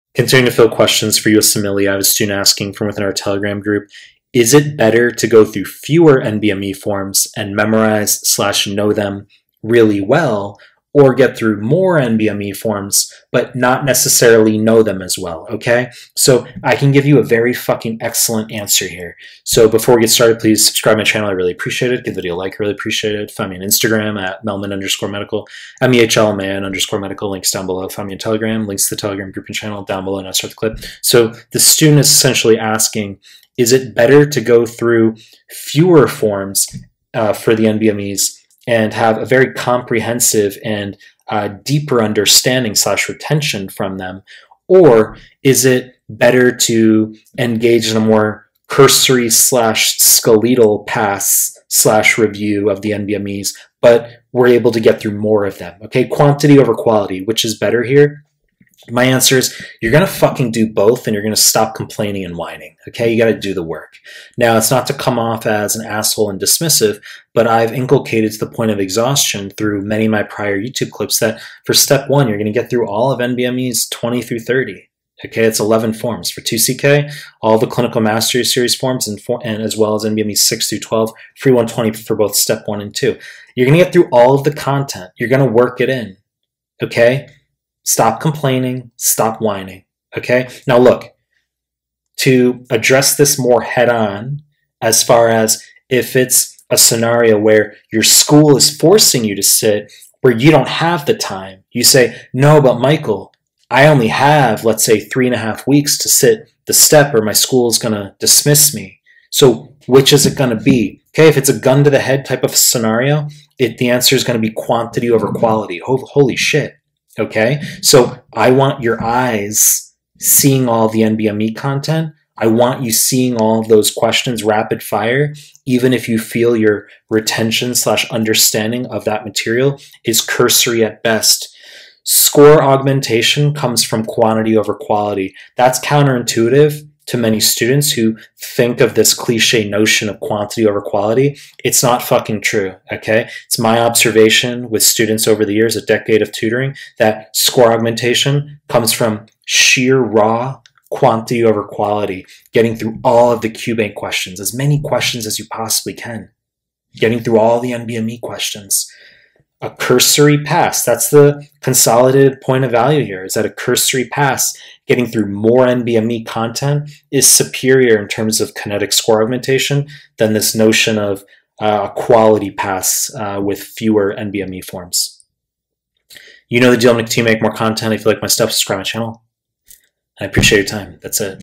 "Which is better - deep review of a few NBMEs, or cursory review of all NBMEs?"
In this clip I answer a student's question about whether reviewing a few NBMEs deeply versus many/all NBMEs more cursory/skeletally is more effective for score augmentation.